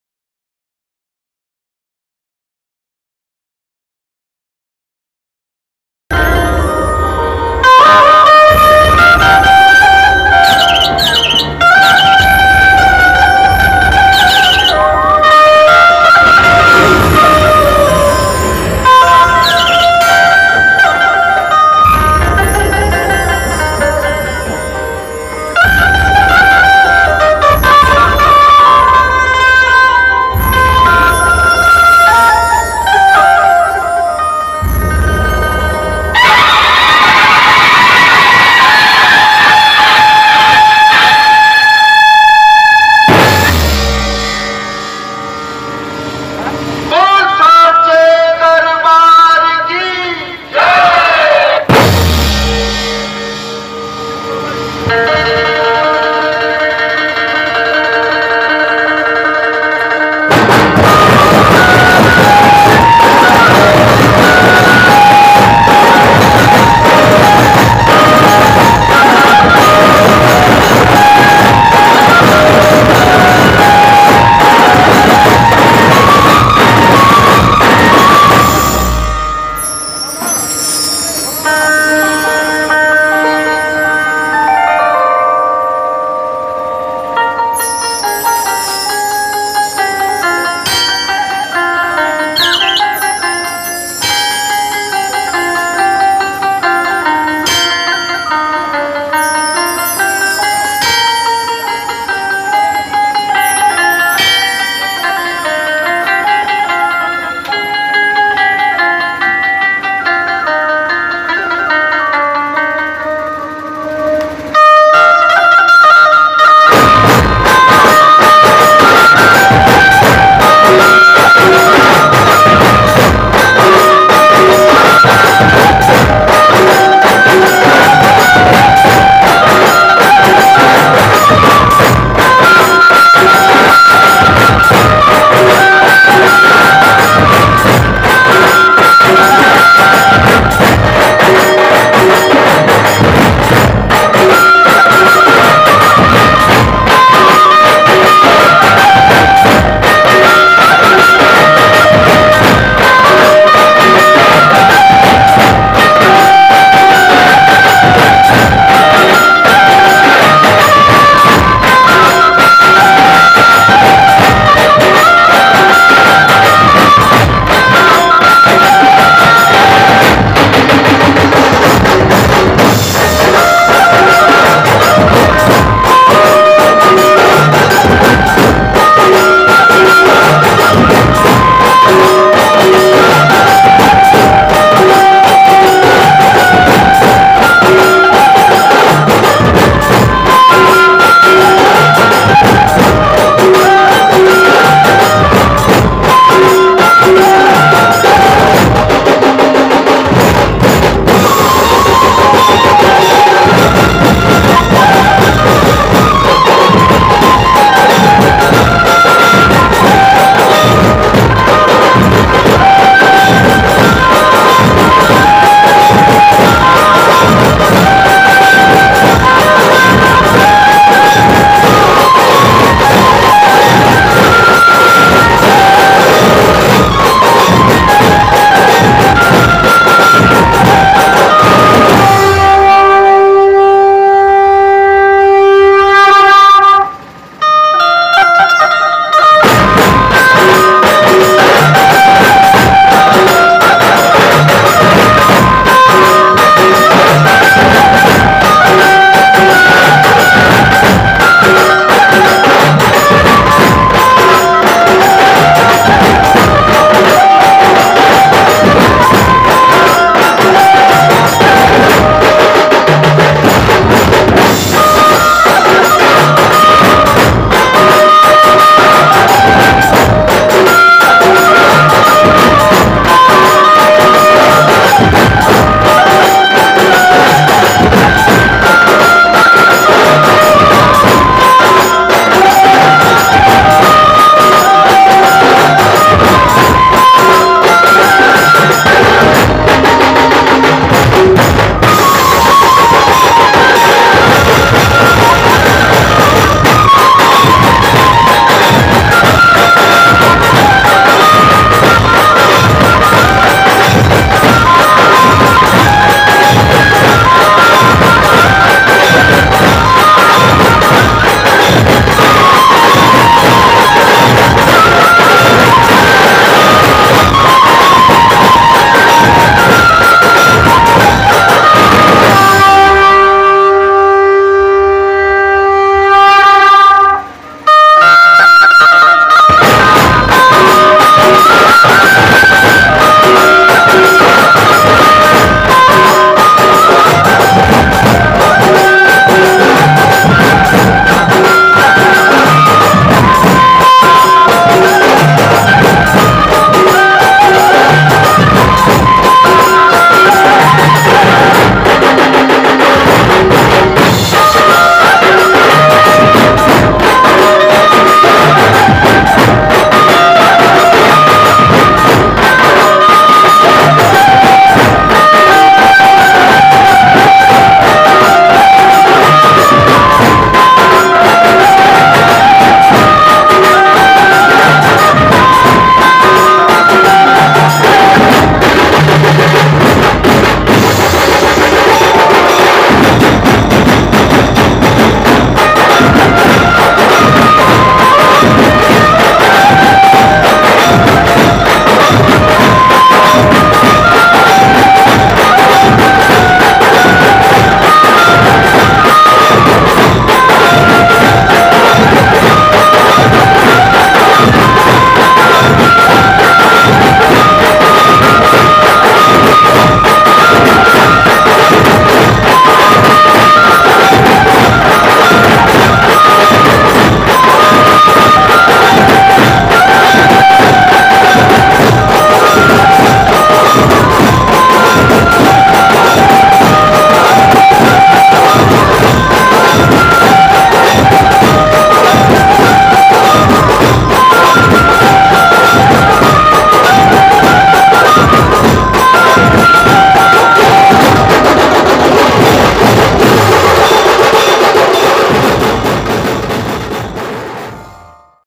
नटराज धुमाल दुर्ग - जवारा विसर्जन रायपुर - माता जी की आरती - BEST PERFORMANCE 2018
NATRAJ DHUMAL PARTY DURG

👌छत्तीसगढ़ का नं. 1 धुमाल चैनल👌